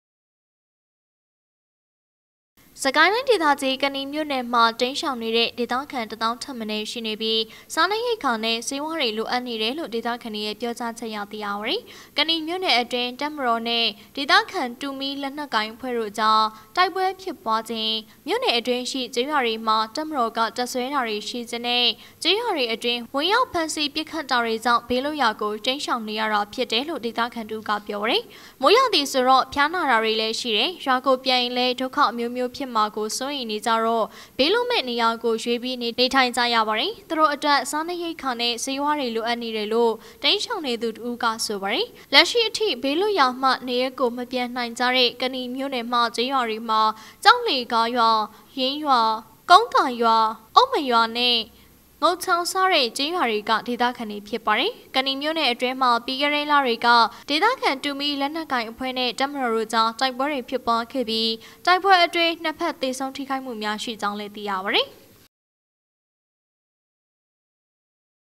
ကနီမြို့နယ်တွင် တိမ်းရှောင်နေရသည့်ဒေသခံ တစ်သောင်းထက်မနည်း ရှိနေ
ကနီမြို့နယ်တွင် တိမ်းရှောင်နေရသည့်ဒေသခံ တစ်သောင်းထက်မနည်း ရှိနေပြီး စားနပ်ရိက္ခာနှင့် ဆေးဝါးများ လိုအပ်နေဟု သိရ
စစ်ကိုင်းတိုင်းဒေသကြီး ကနီမြို့နယ်မှာ တိမ်းရှောင်နေရတဲ့ ဒေသခံ တစ်သောင်းထက်မနည်းရှိနေပြီး စားနပ်ရိက္ခာနဲ့ ဆေးဝါးတွေ လိုအပ်နေတယ်လို့ ဒေသခံတွေရဲ့ ပြောကြားချက်အရ သိရပါတယ်။